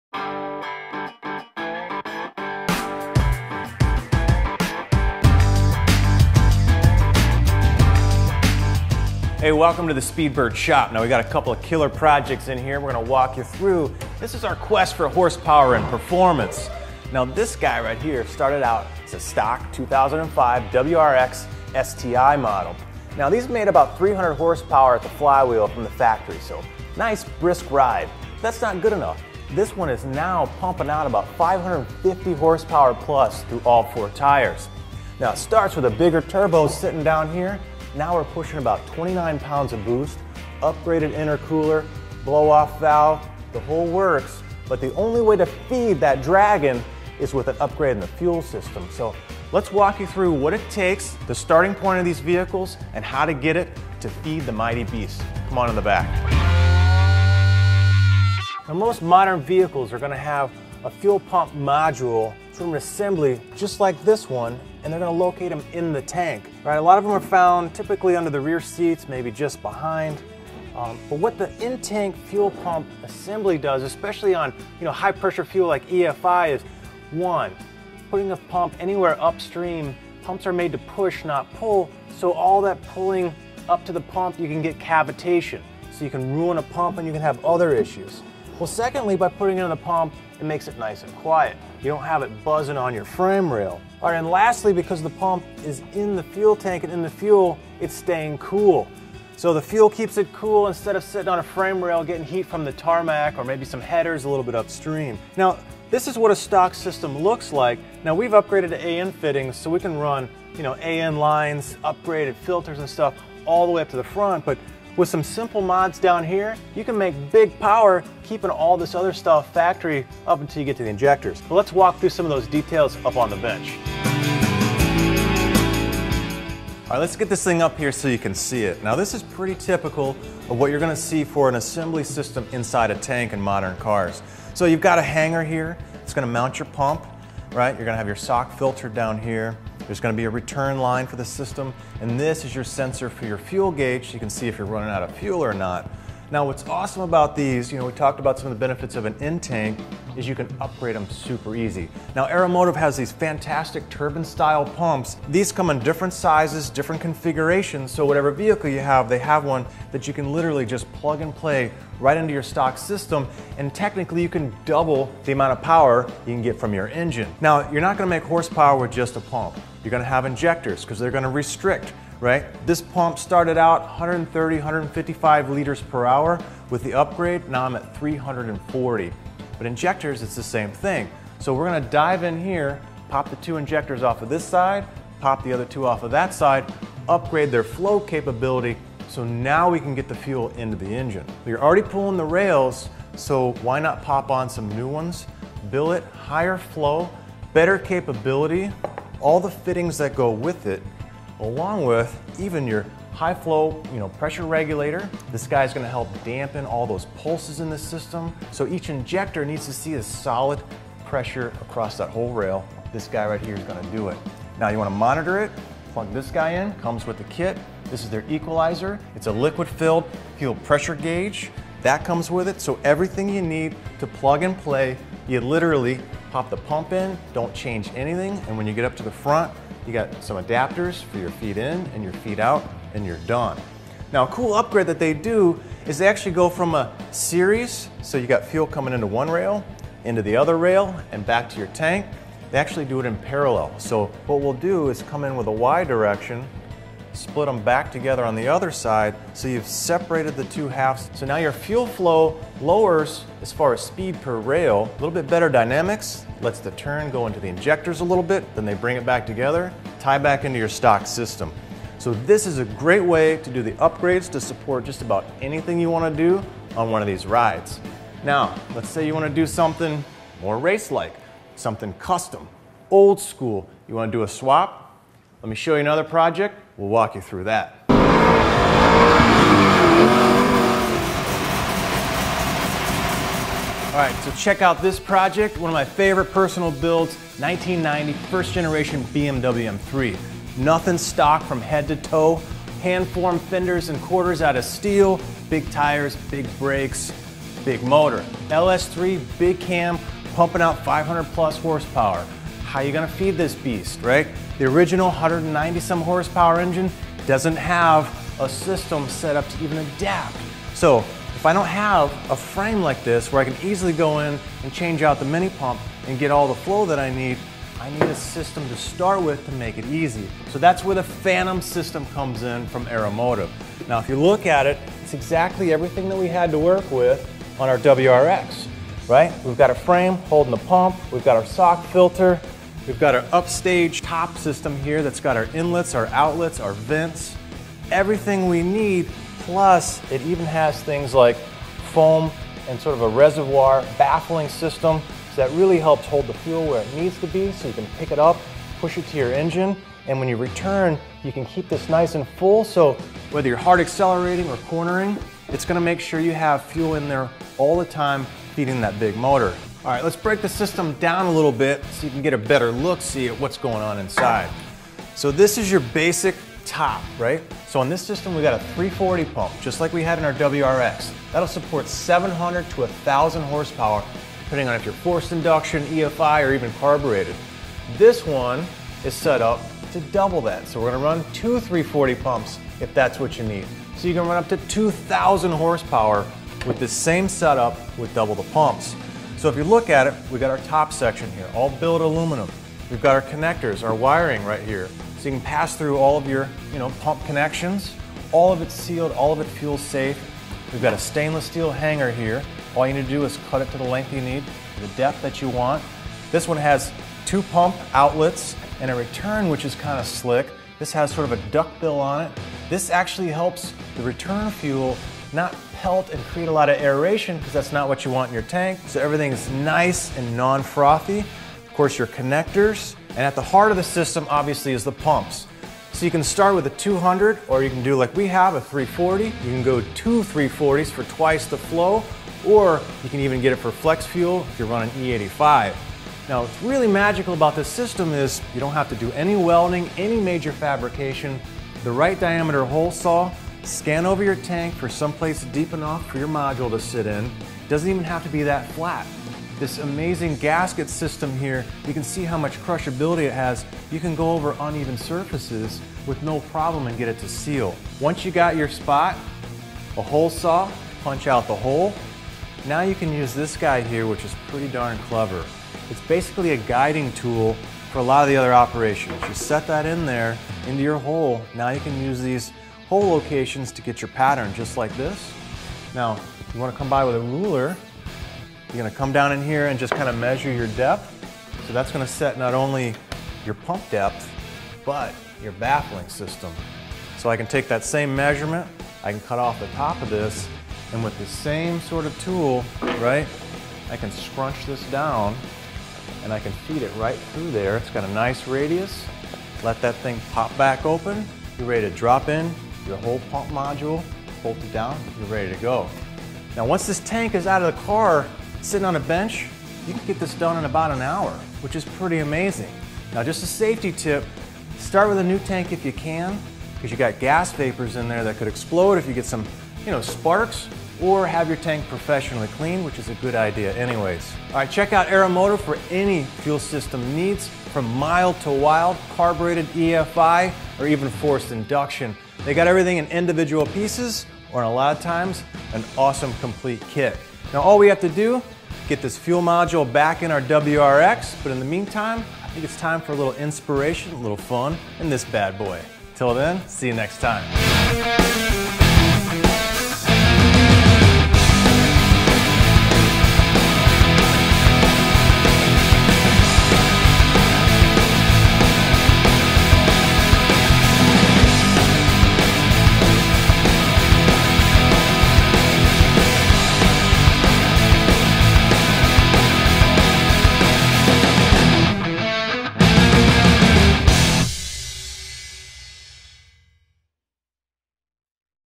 Aeromotive Fuel System Overview
Watch as Kevin Byrd walks us through the Aeromotive in-tank fuel pump upgrade on a WRX as well as the installation of the Phantom Fuel Pump kit on a custom application.